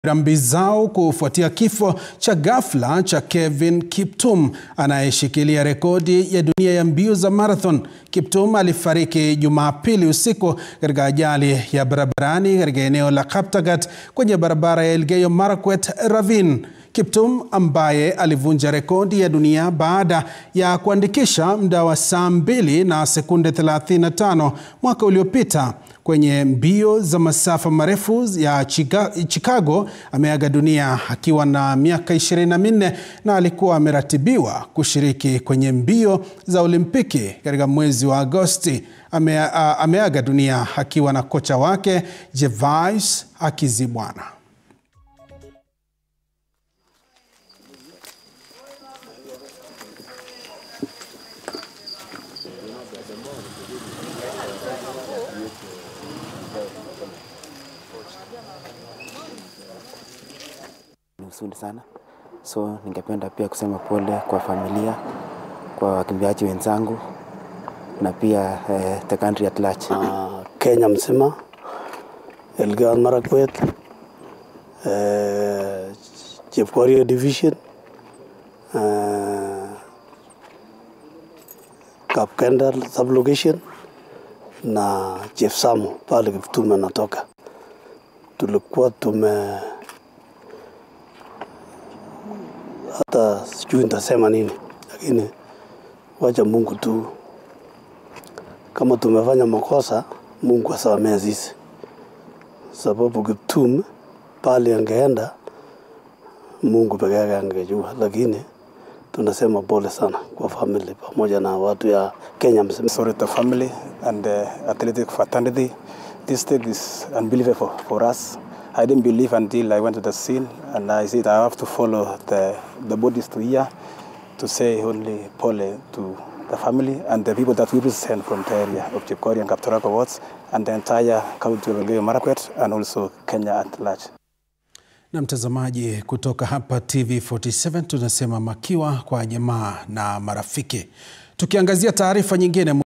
Mshikilizi wa mbio za dunia za masafa marefu Kelvin Kiptum aaga dunia
Viongozi nchini wametoa rambirambi zao kufuatia kifo cha ghafla cha Kelvin Kiptum, anayeshikilia rekodi ya dunia ya mbio za marathon.

Kiptum alifariki jumapili usiku katika ajali ya barabarani huko kaptagat kwenye barabara ya Elgeyo Marakwet-Ravine.

Kiptum ambaye alivunja rekodi ya dunia baada ya kuandikisha muda wa masaa 2 na sekunde 35 mwaka uliopita kwenye mbio za masafa marefu za Chicago ameaga dunia akiwa na miaka 24 na alikuwa ameratibiwa kushiriki kwenye mbio za Olimpiki katika mwezi wa Agosti.

Ameaga dunia akiwa na kocha wake Gervais Hakizimana.